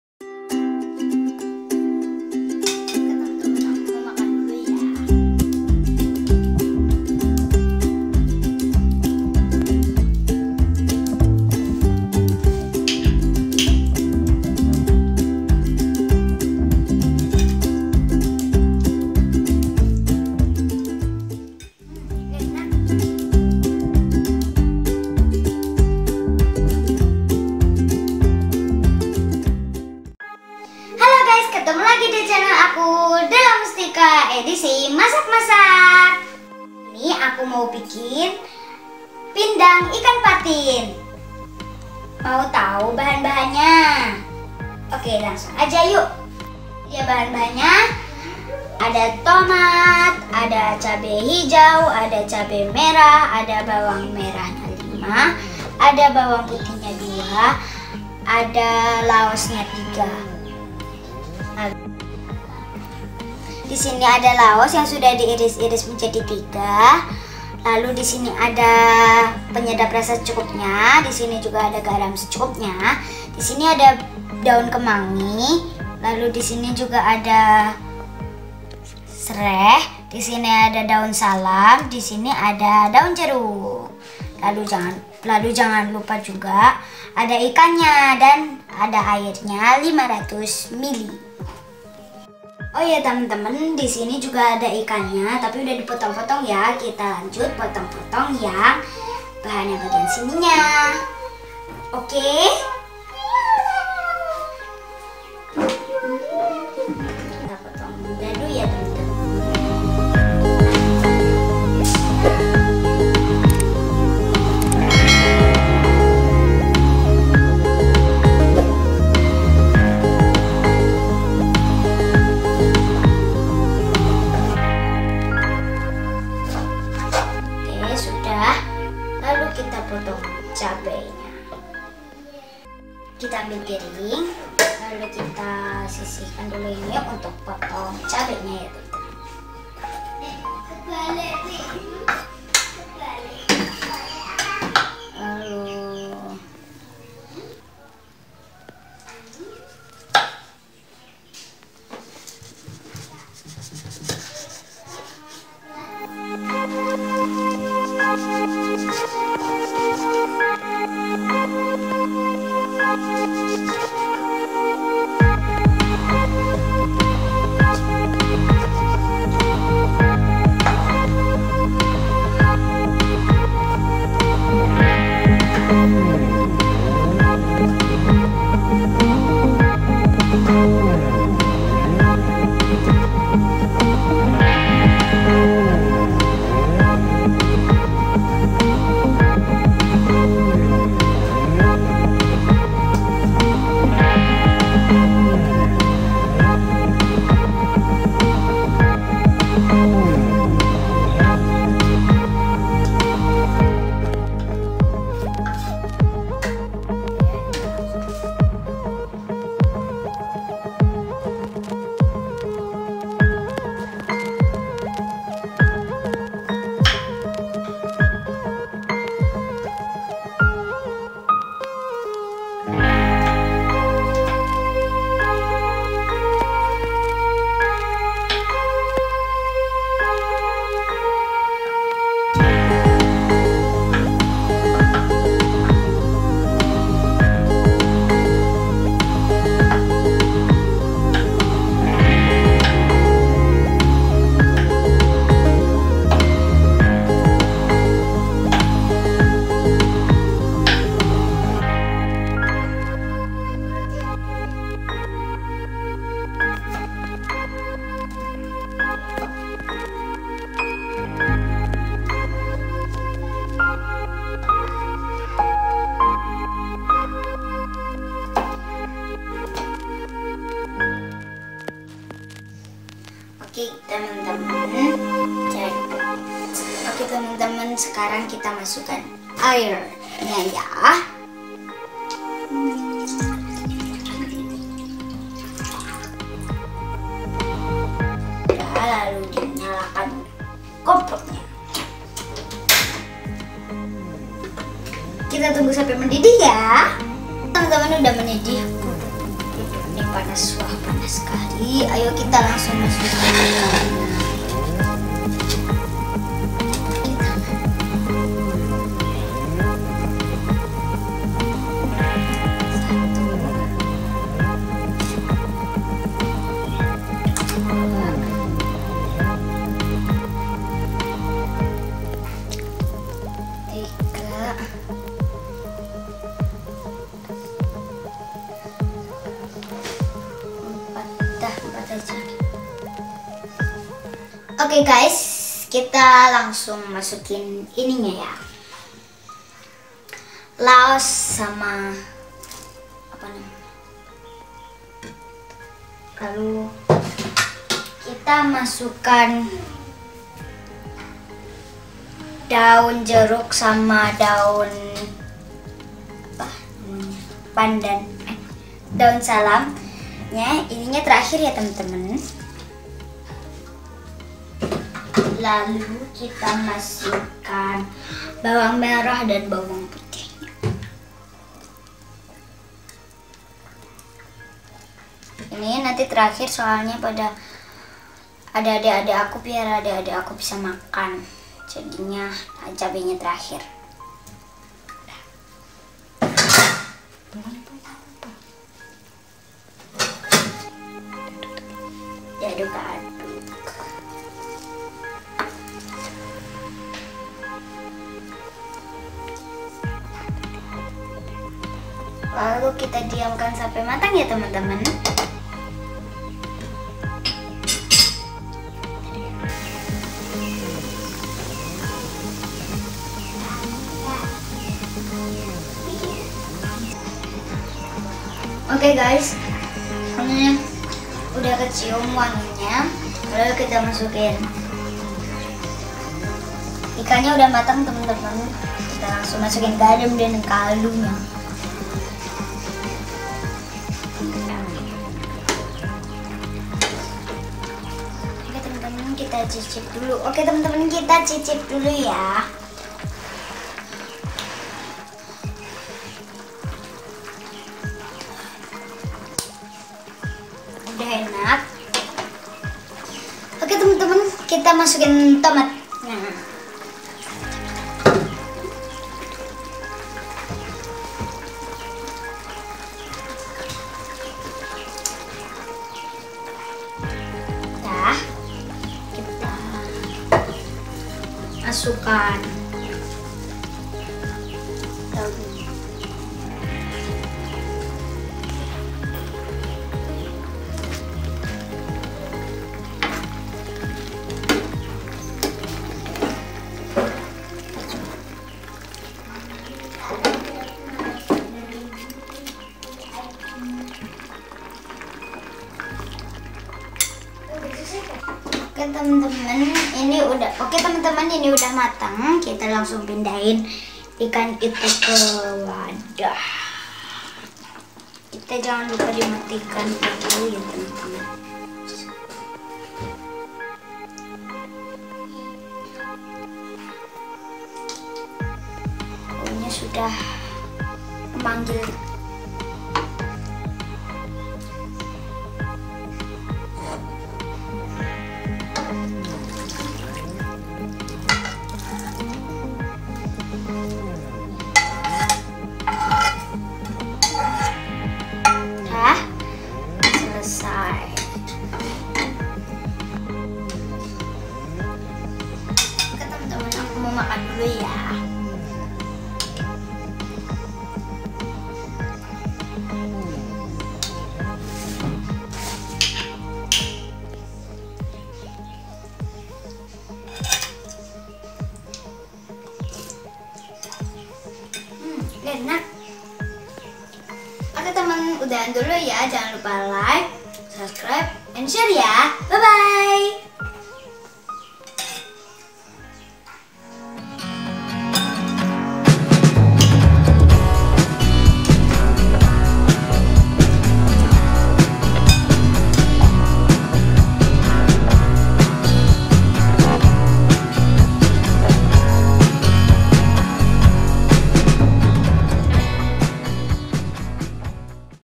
Resep Pindang Ikan Patin ala Kak Della - Pindang Ikan Patin Khas Palembang
Assalamualaikum teman-teman...
Di vidio aku kali ini, aku ingin berbagi resep salah satu makanan pavorit aku juga loooh, namanya PINDANG IKAN PATIN. mungkin kalo dilihat dari banyaknya bahan kalian akan pusing tapi kalo udah di praktekin cara buatnya mudah banget loh,,, Makanya ikutin terus vidio aku sampai selesai ya... ingat caten juga bahan-bahannya okeeee